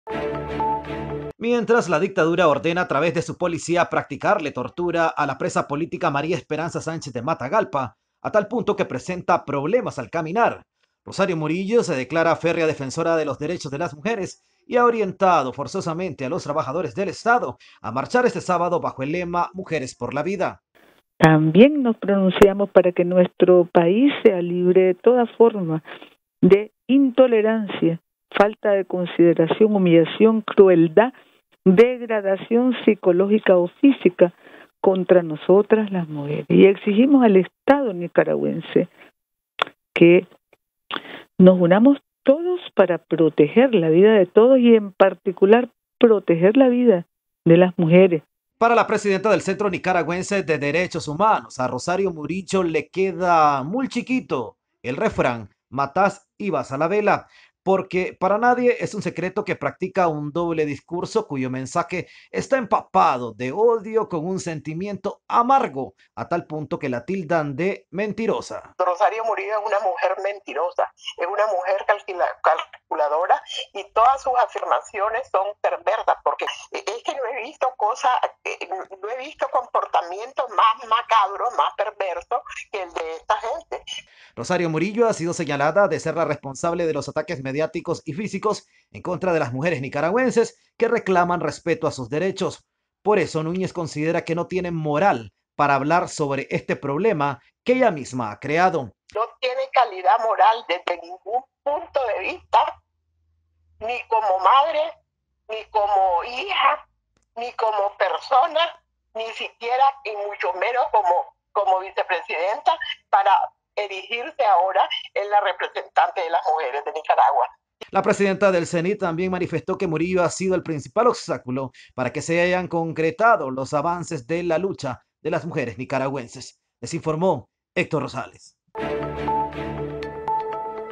Vilma Núñez: “El refrán MATÁS Y VAS A LA VELA, le queda muy chiquito a Rosario Murillo”
Mientras la dictadura ordena a través de su policía a practicarle tortura a la presa política María Espinoza Sánchez de Matagalpa, a tal punto que presenta problemas al caminar, Rosario Murillo se declara férrea defensora de los derechos de las mujeres y ha orientado forzosamente a los trabajadores del Estado a marchar este sábado bajo el lema de “Mujeres por la vida”